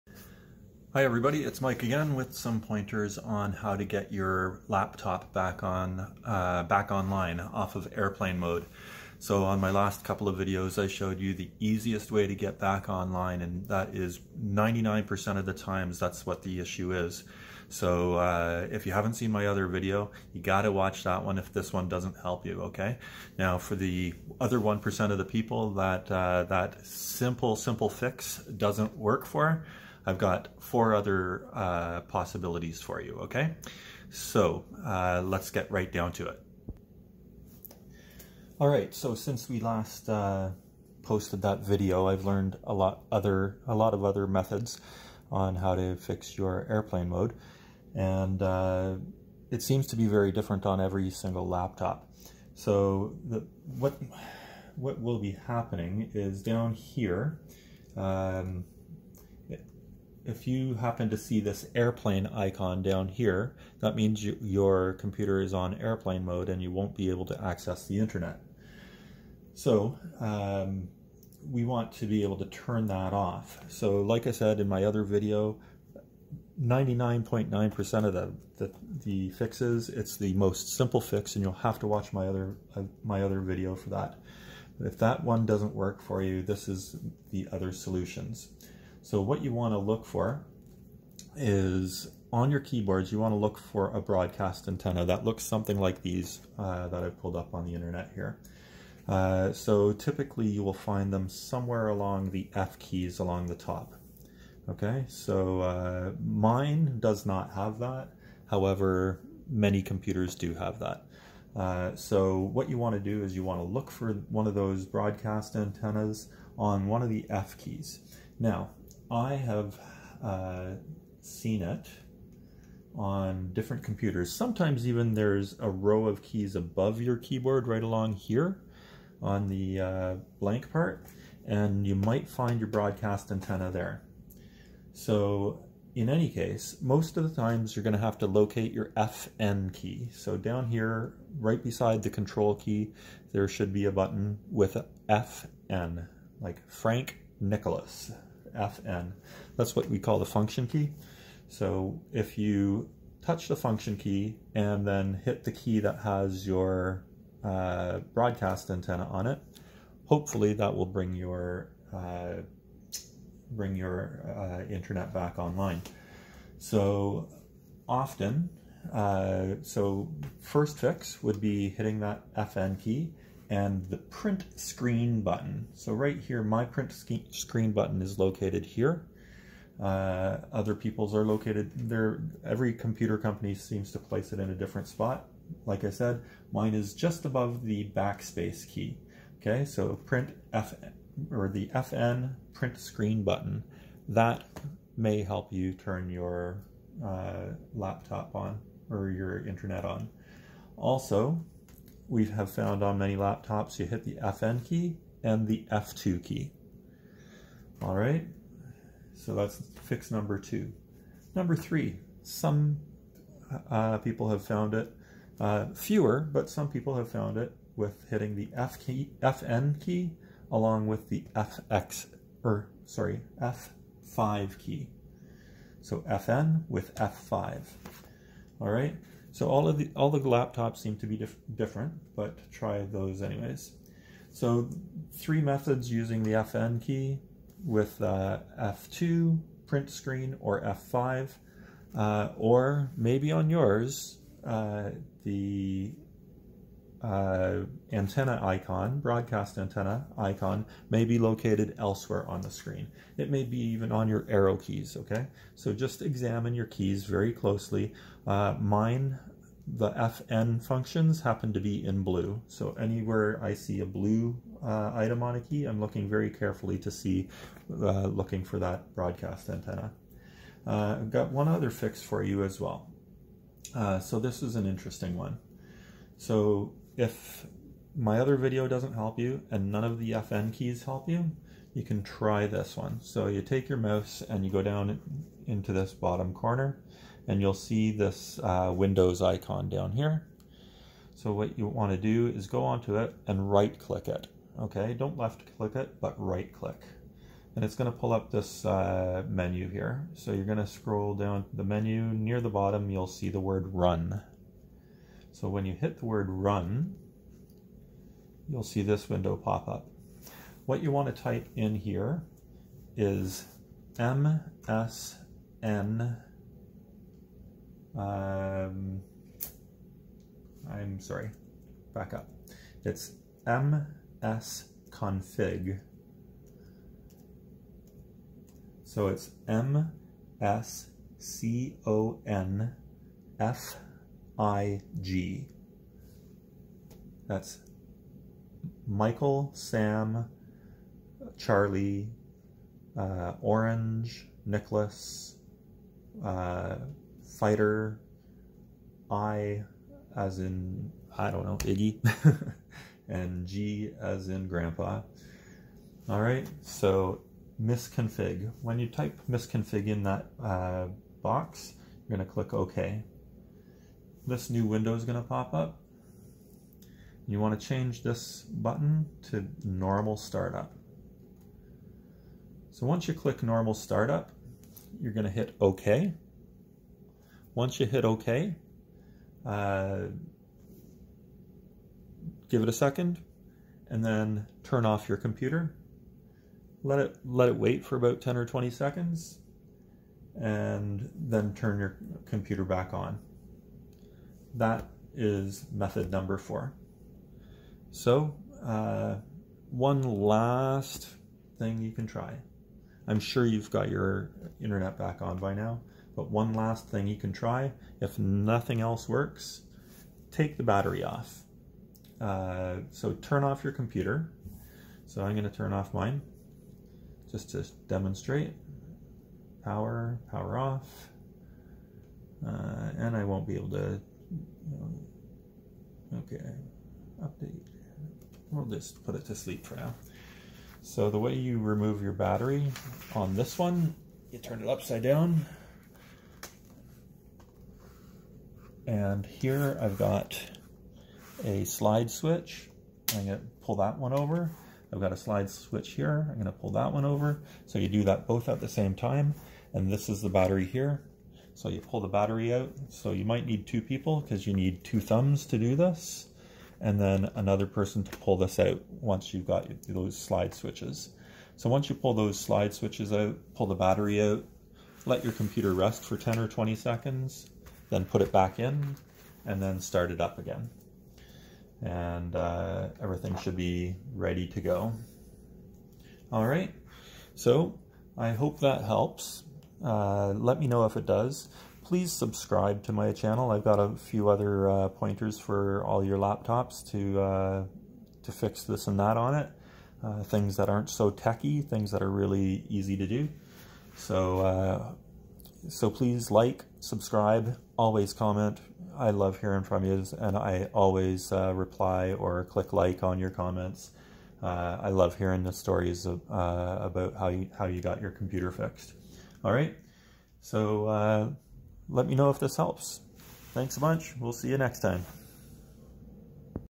5 fixes to turn off airplane mode on laptops.
5 easy ways to turn off airplane mode on laptops.

This video is the solution for the following ERROR MESSAGE:
"You're not connected and the web just isn't the same without you. Let's get you back online. Check that your network cables are plugged in.  Make sure you're not in airplane mode see if your wireless switch is turned on. Restart your router."
OR
"No internet. Try: checking the network cables, modem and router.  Reconnecting to wifi. Running windows network diagnostics.  err_internet_disconnected